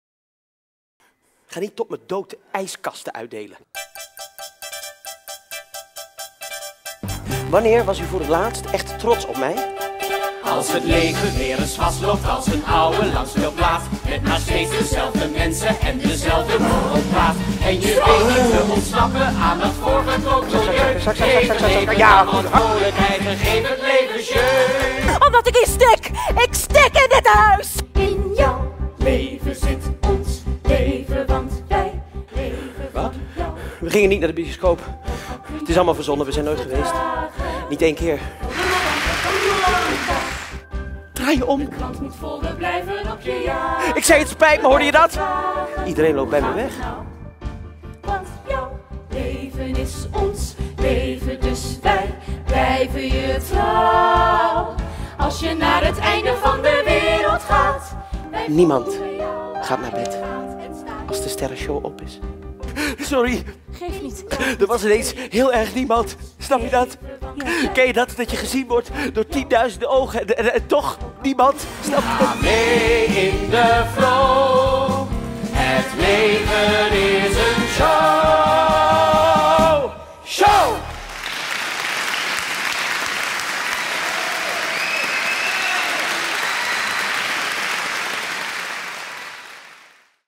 Willem Ruis de show van zijn leven, Trailer
Met scenes uit de voorstelling.
Nu in het theater tot 14 maart 2016.